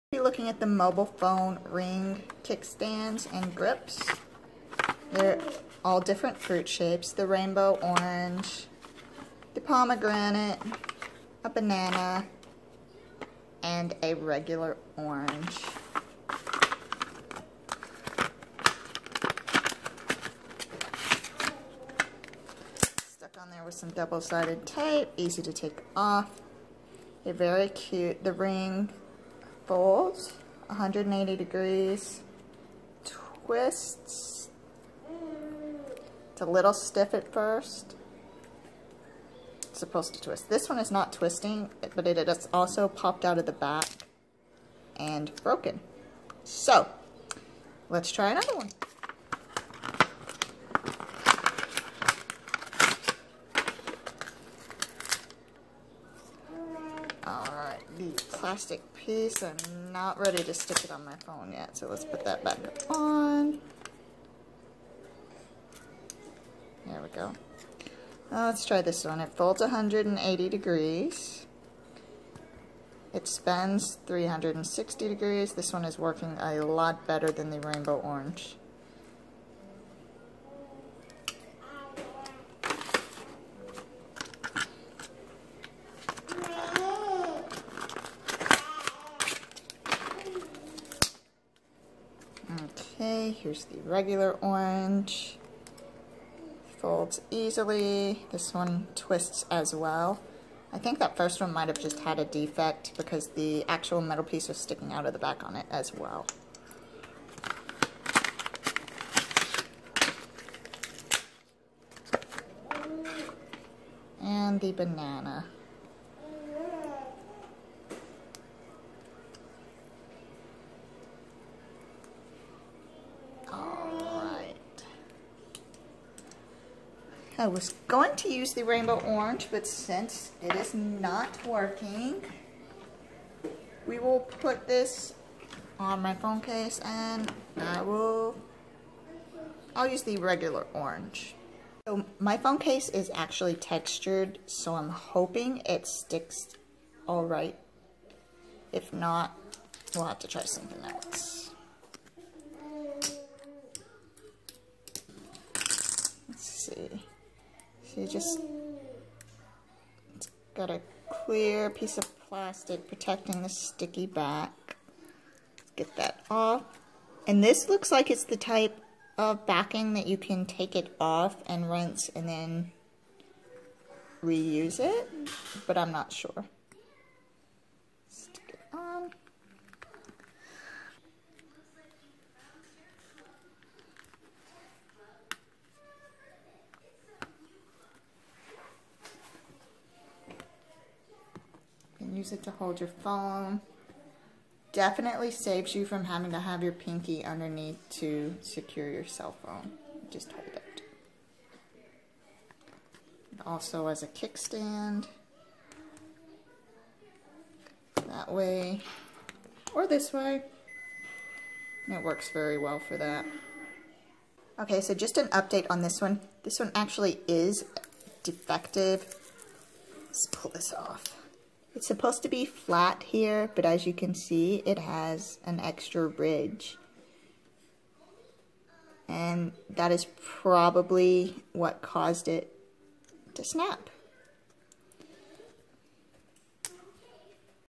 Maeffort Cell phone holder, 4 Pack Universal Smartphone Ring Grip Stand
Cute quirky designs. These kickstand rings are bright and vibrant. The cute designs paires with functionality make this a win win.

The ring stick very well. I had to tug very hard to get them to come off which is not going to happen in every day life. They're reusable as well so if you happen to get a new phone or case you can simply remove them then wash and re-stick, which is awesome. Thr kickstand aspect of it is great for watching movies or shows. it works vertically as well as horizontally.

I particularly love that I do not drop my phone anywhere near as much as I had without using a ring.

I did have any issue with one  of my rings. My favorite, the rainbow orange, had a defect and smapped in half during my video before I could even try it out. It would never have stuck to my case flush because of the defect. Hopefully this is an odd occurrence rather than standard.

I will also note that the description states not to use on textured surfaces, but it worked fine on my case. Also the title states it is a car mount. It does not come with the hook for your vehicle. I have one from a previous purchase so that was not an issue for me.